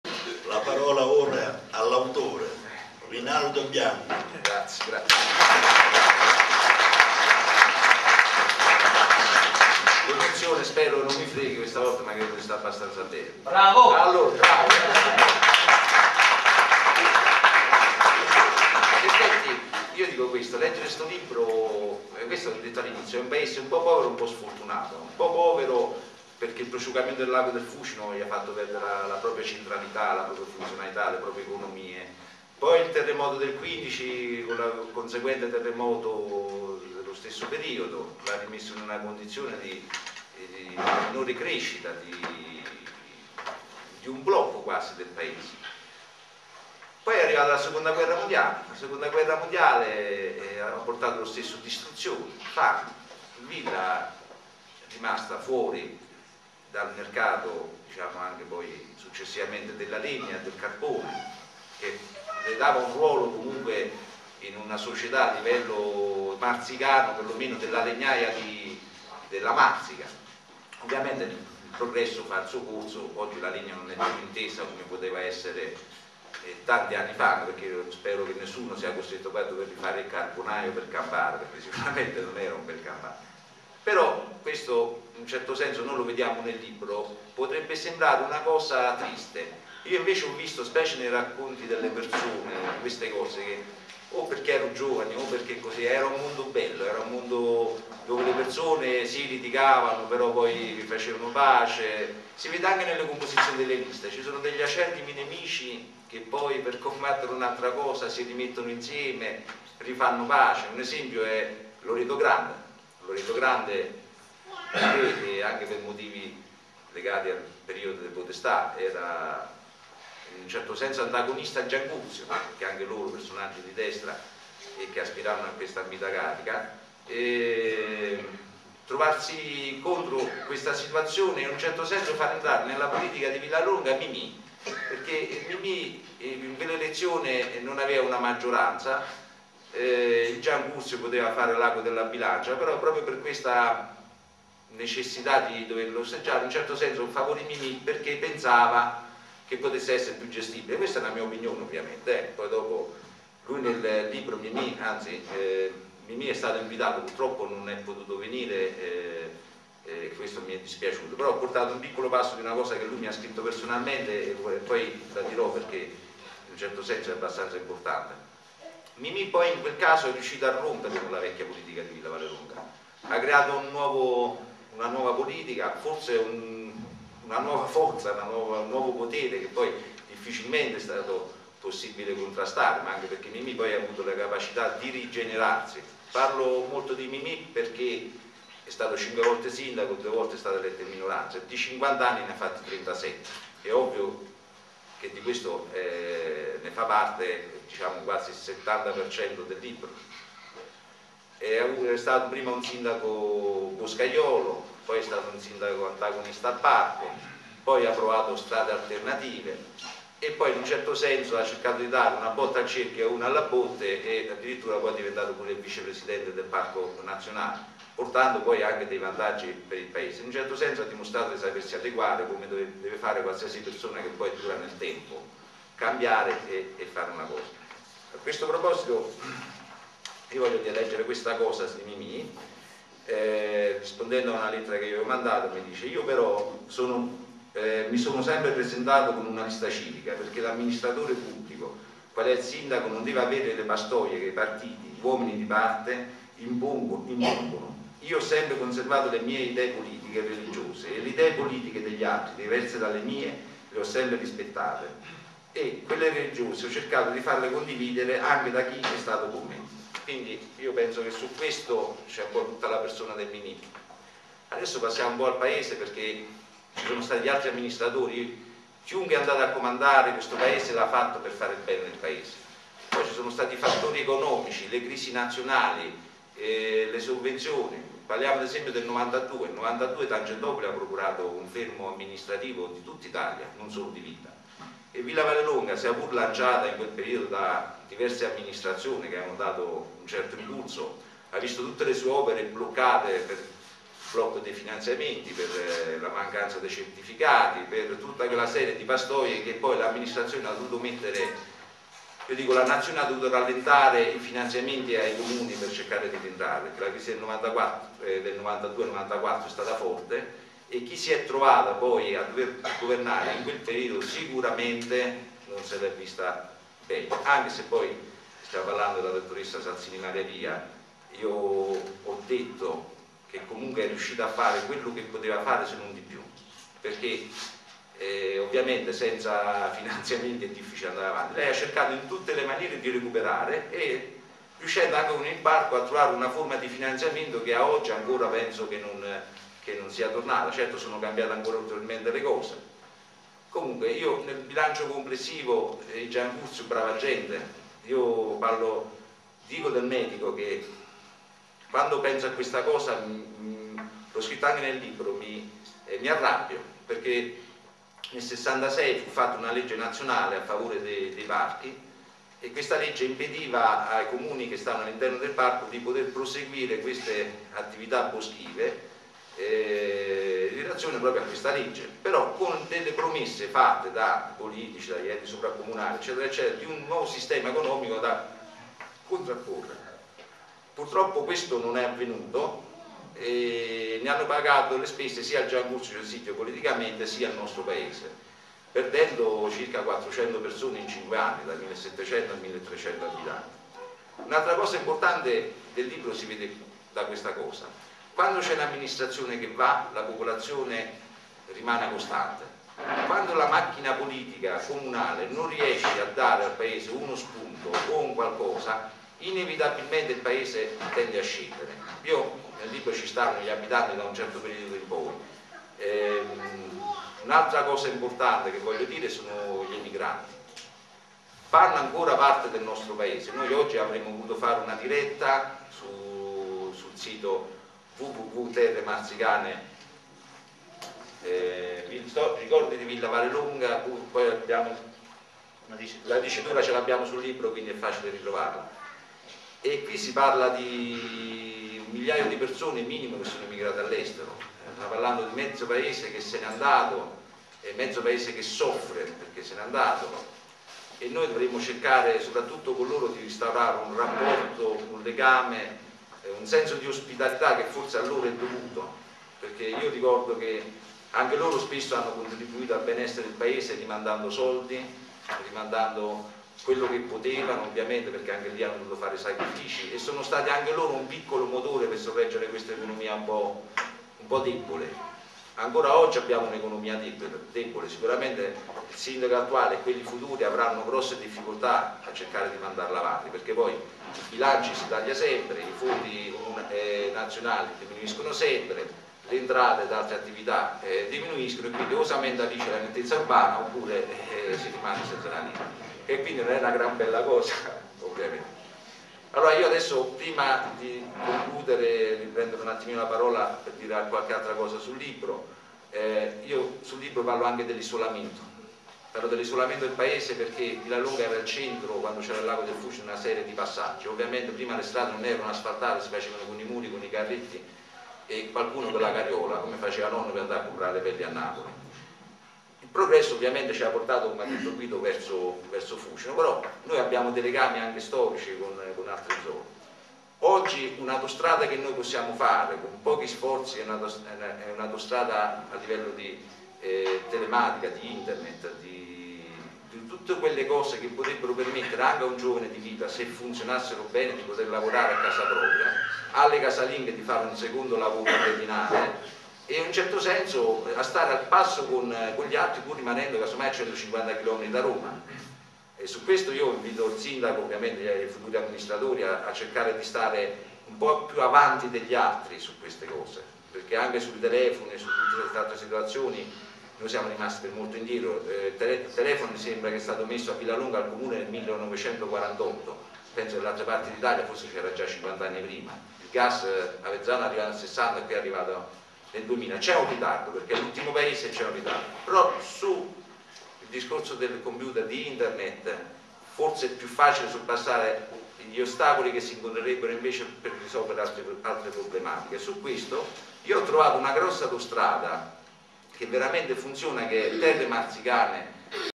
"Ricordi di Villavallelonga" (4)
Villavallelonga (AQ) Presentazione del libro "Ricordi di Villavallelonga" di Bianchi  Rinaldo Intervento dell'Autore Rinaldo Bianchi